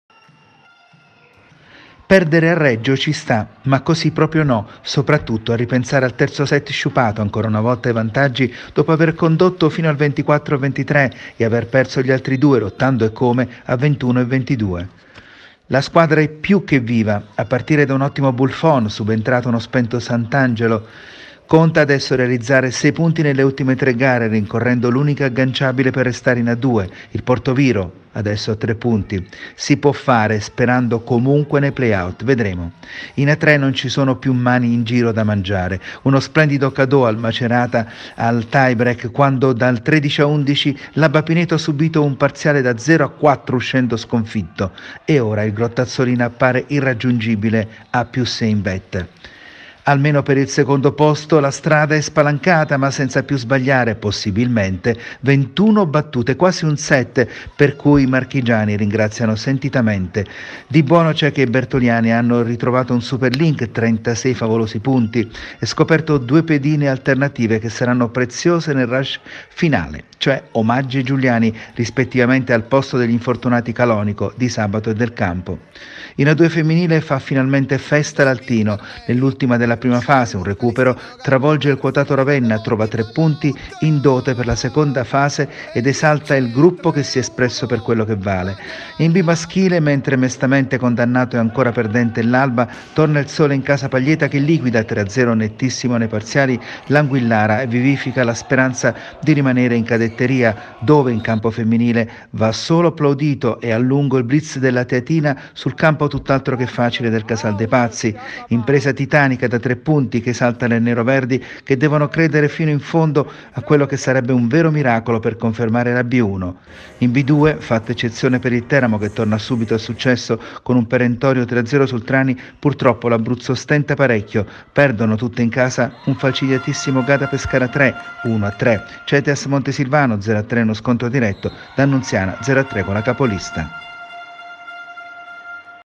Volley. Sieco e Abba ko, primo sorriso per l’Altino
Volley A2. La sconfitta di Reggio Emilia complica i piani salvezza per la Sieco Ortona che ora si gioca tutto nelle ultime 3 giornate. In A3 l’Abba Pineto cade al tie break con Macerata e si stacca dal primo posto. In A2 femminile l’Altino trova la prima vittoria proprio nell’ultima giornata di regular season